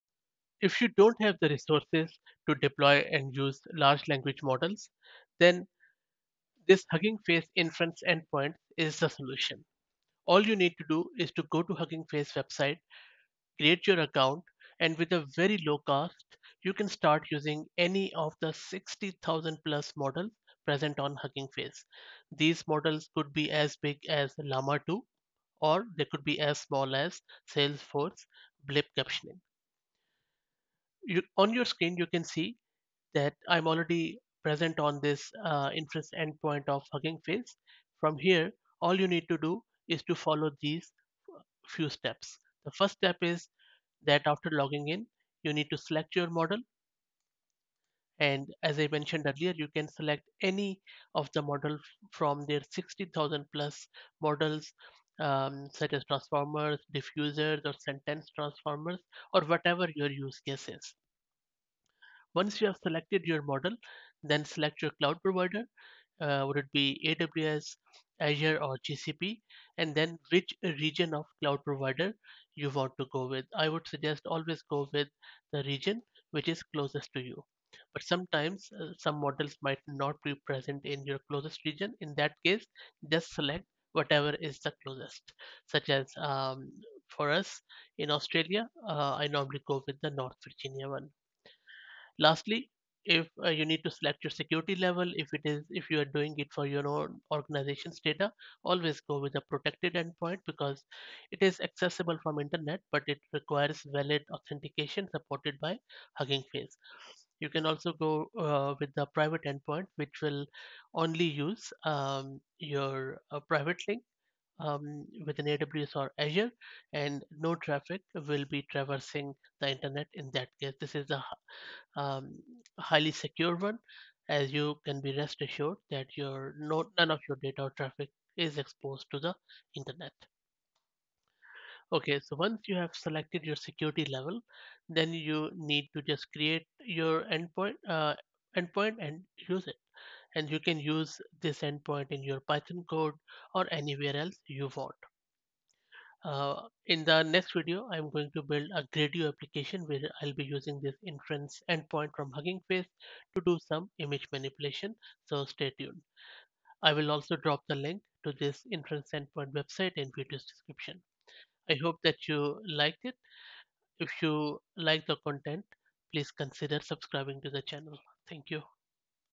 What are Huggingface Inference Endpoints
With Inference endpoints, you can deploy a custom model or any of the 60,000+ Transformers, Diffusers or Sentence Transformers models available on the huggingface.